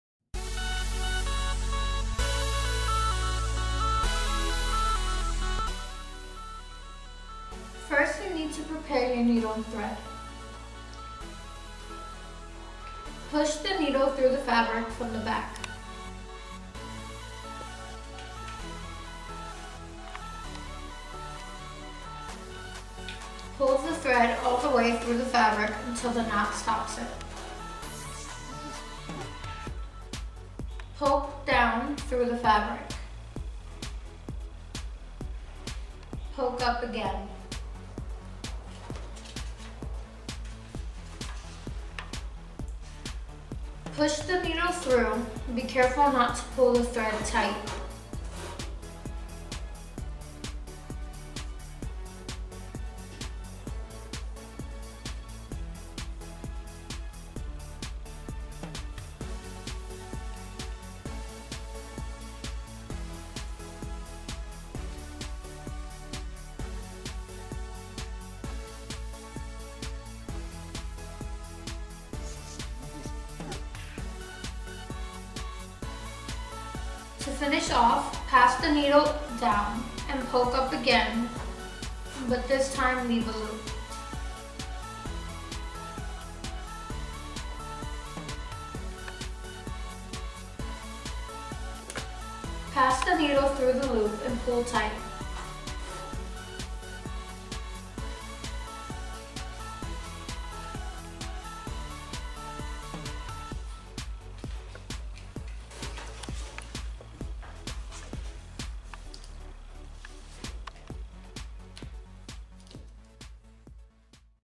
Designer 1 Sewing Skills | How to Hand Sew Basting Stitches
Improve your sewing skills with these step by step instructions.

Skills Workshop: Hand sewing - basting stitch

Now that we’ve prepared our needle and thread let’s learn our first stitch, the basting stitch. BASTING STITCHES ARE LARGE, LOOSE STITCHES. They are commonly used to temporarily hold fabric together or mark the location for a stitching line before making more permanent stitches by hand or with a sewing machine.

To begin, first you need to prepare your needle and thread.

Push the needle through the fabric from the back. Pull the thread all the way through the fabric until the knot stops it. Poke down through the fabric. Poke up again. Push the needle through. Make sure not to pull the fabric tight. To finish off pass the needle down and push up again but this time leave a little room. Pass the needle through the loop and pull tight. Secure the stitch.